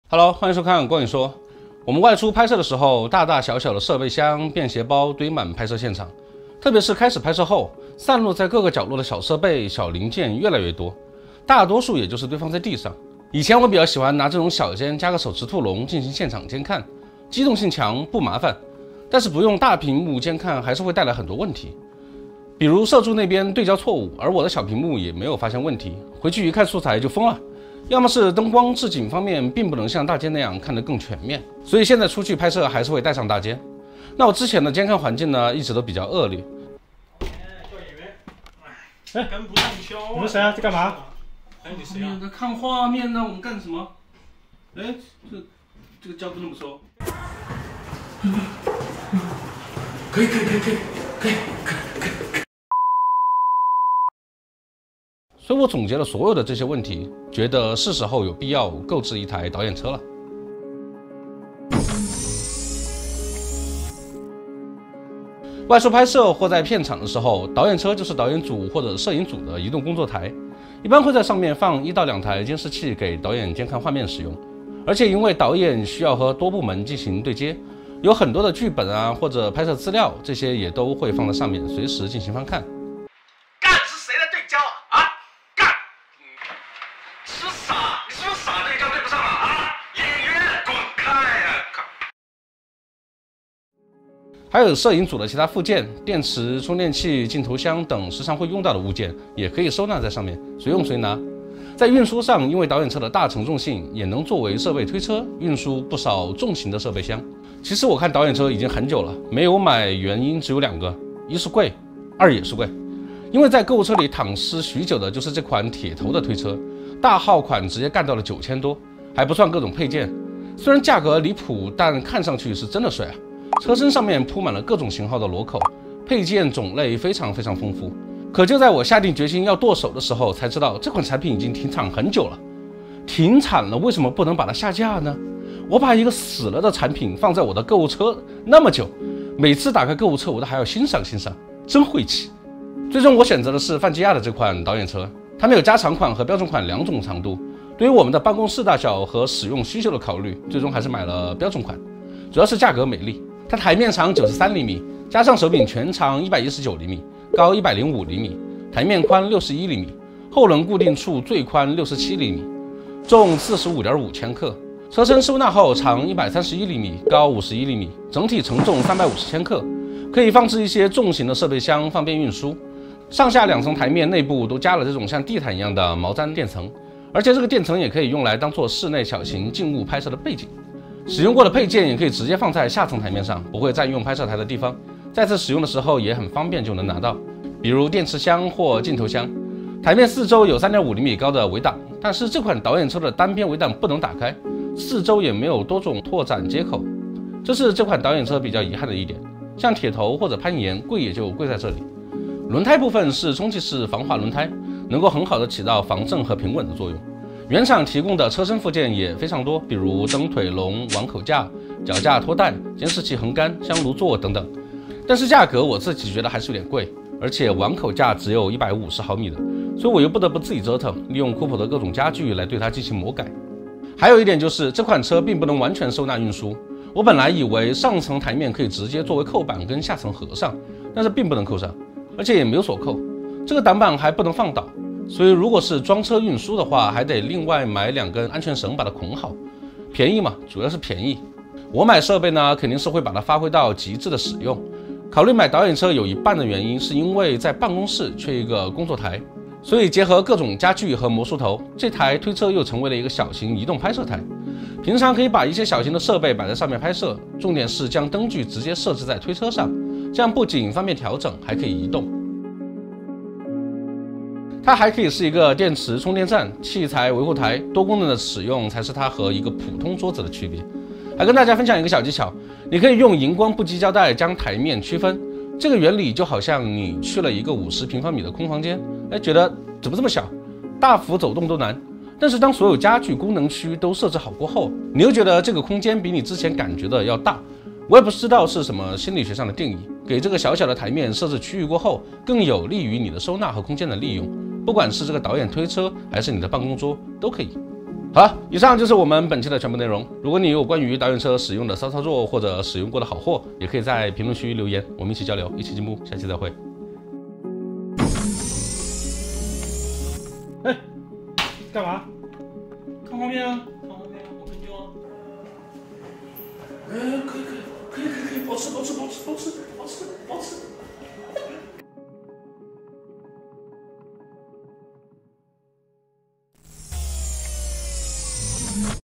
【光影说】为什么拍摄需要一台导演推车？
欢迎观看【光影说】，本期将为大家介绍我们自用的Filmgear泛基亚导演推车，还希望大家跟我们一起交流，一起进步！
本期拍摄使用设备：
摄影组：bmpcc6kpro，DZOfilm玄蜂Vespid电影镜头套装，DZOFilm观系列微距电影镜头套装，致迅影眸图传，青峰电影三脚架，青牛BX300，铁头Tilta原力M跟焦系统，BESTVIEW S21监视器，艾肯Portkey LH5P监视器，Filmgear导演车。
灯光组：思锐E30b，爱图仕60x，爱图仕MC，爱图仕B7C，爱图仕F22c，南光pavotube30c，南光60c，锐鹰18TDXii，KUPO KT2436旗板套装，世光Sekonic L-478D测光表，Kupo 186f 灯架，Kupo 135灯架，Kupo155灯架，meking美铿MFA-4500灯架，Kupo 20” 魔术腿，不知名魔术腿
声音组：Zoom F6，索尼Sony D21无线麦克风套装。